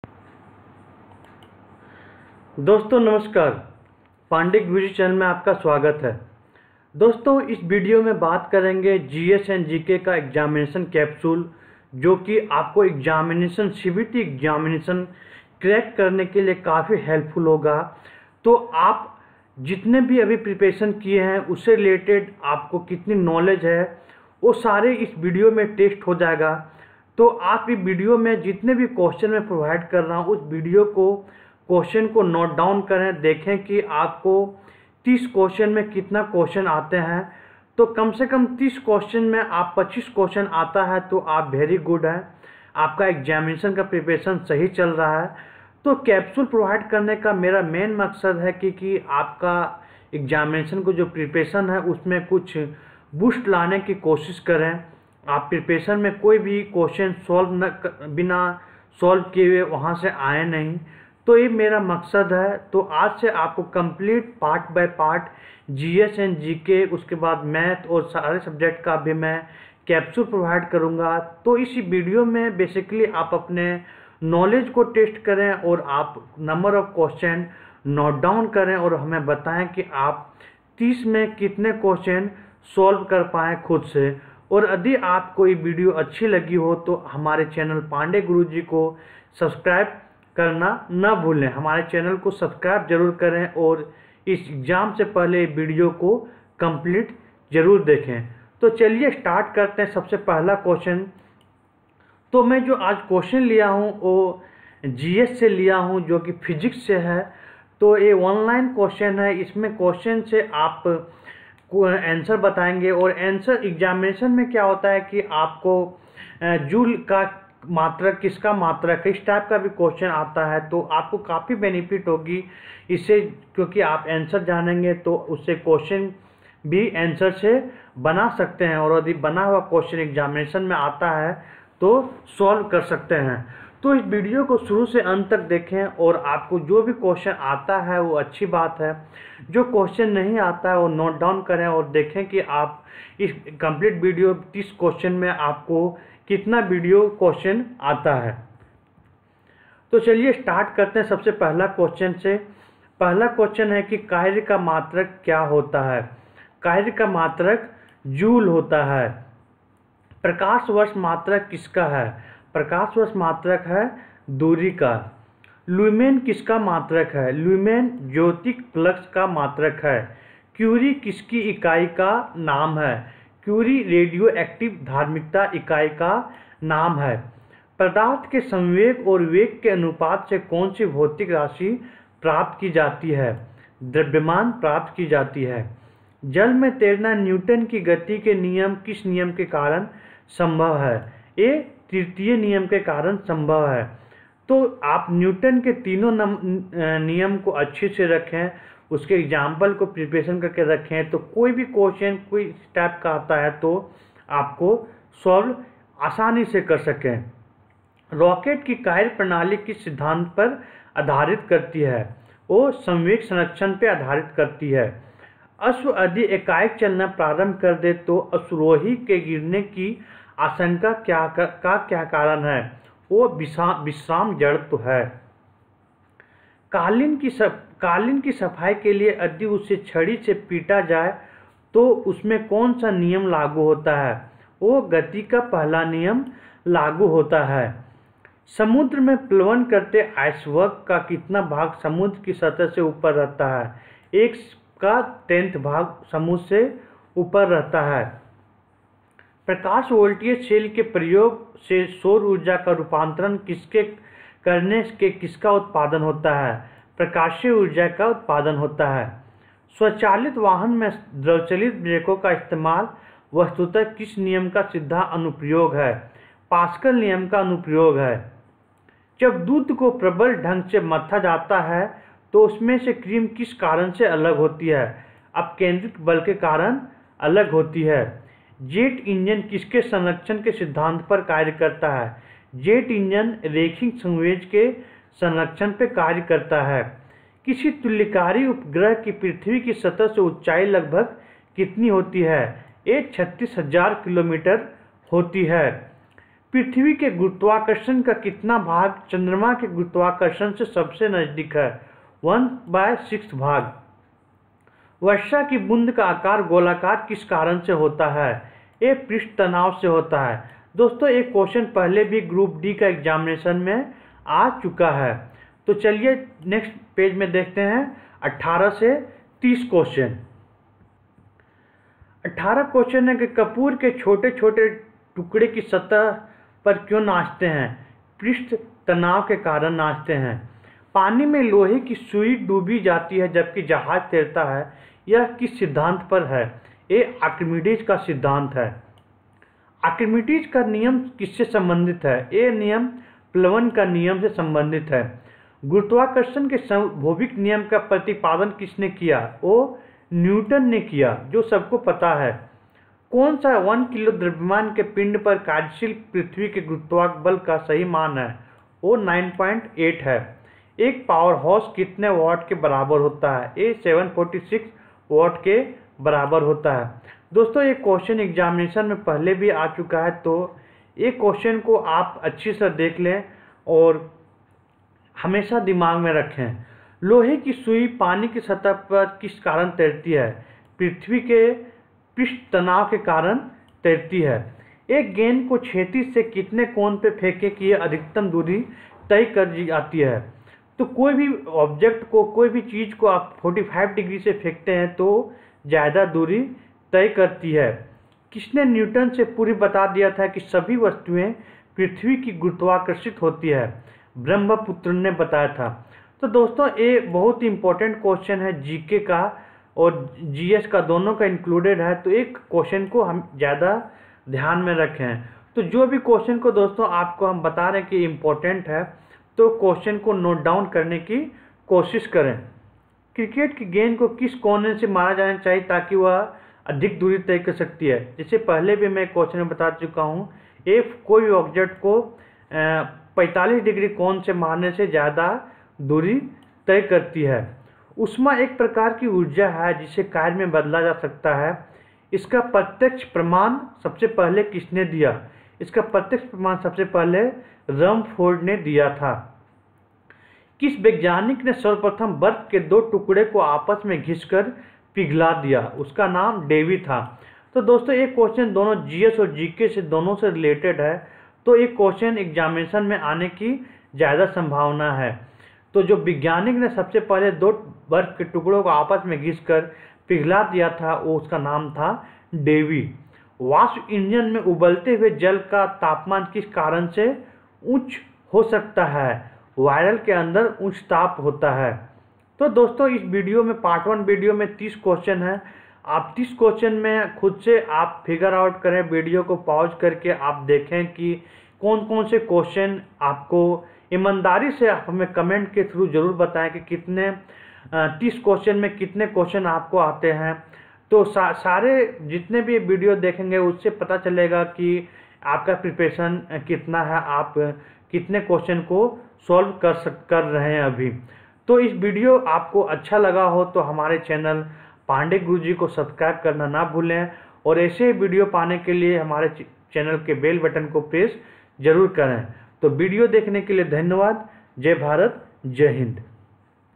Railway group d exam preparation|| GS & GK capsule Part-1
Railway group d exam preparation in Hindi
General science and General knowledge capsule.
CBT पास करने का  कैप्सूल